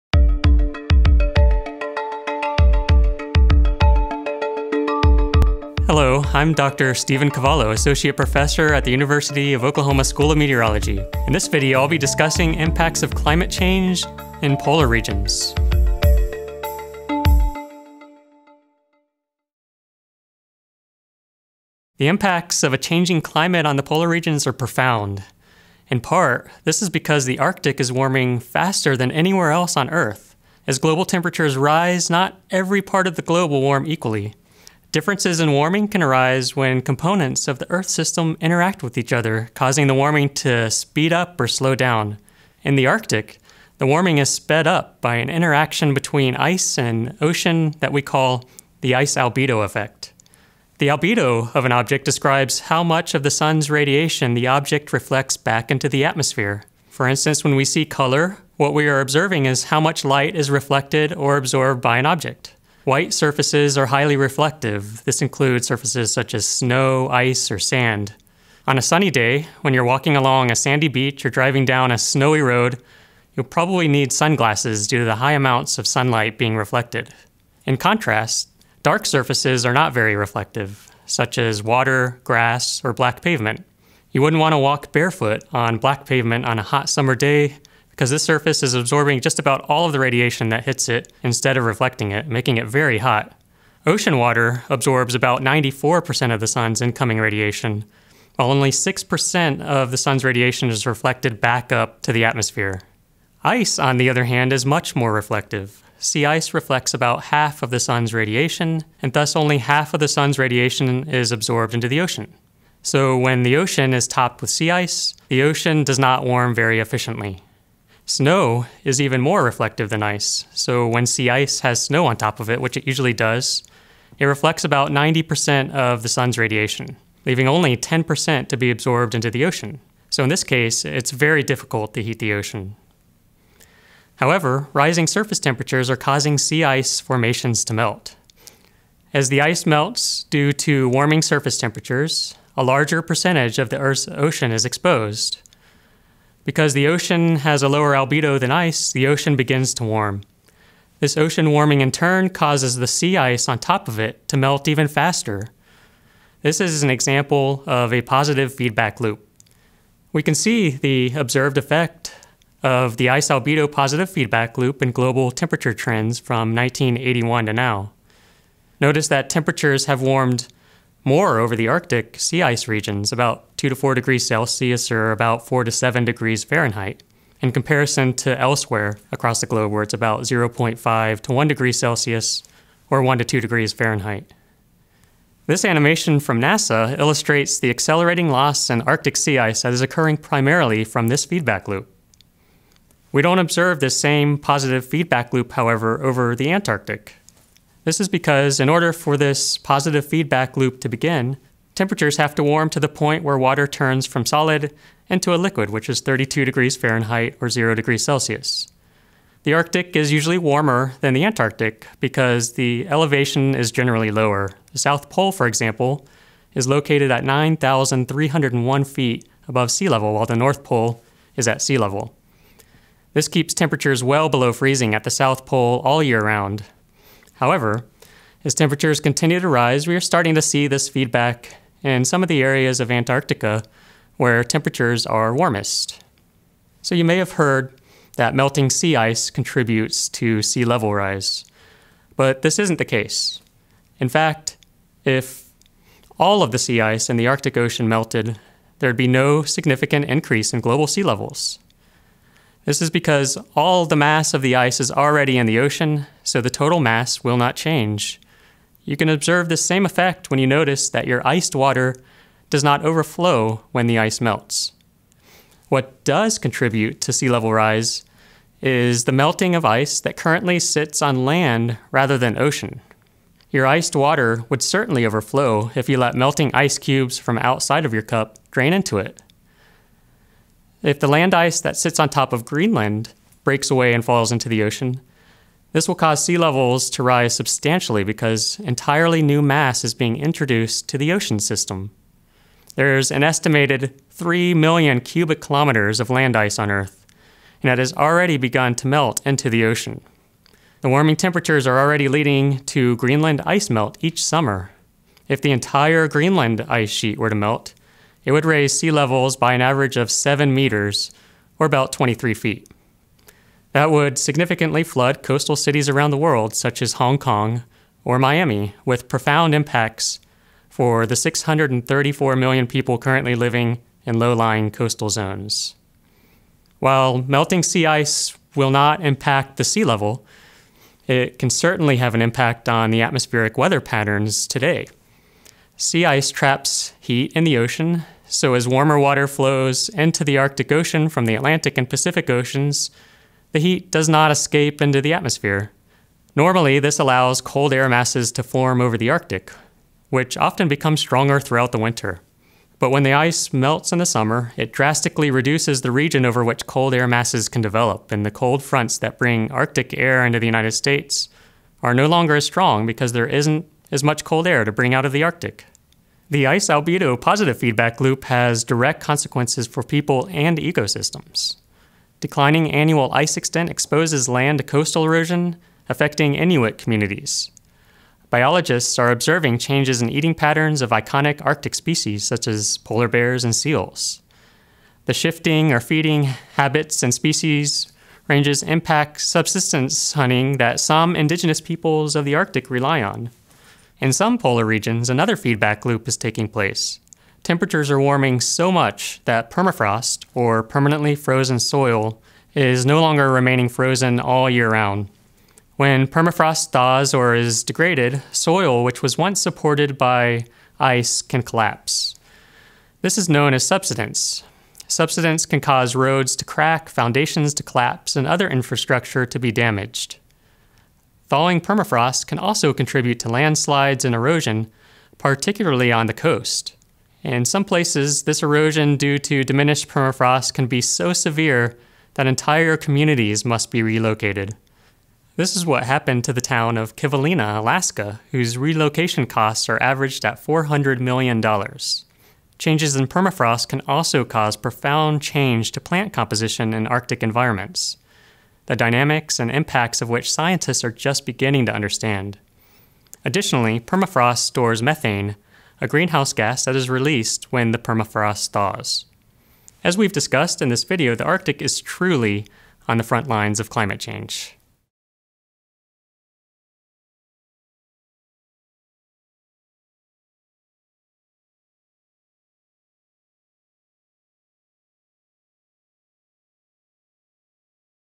Impacts to polar regions
Managing for a Changing Climate is a course offered by the University of Oklahoma.

This video discusses climate impacts on polar regions.

Guest Instructor: Steven Cavallo, University of Oklahoma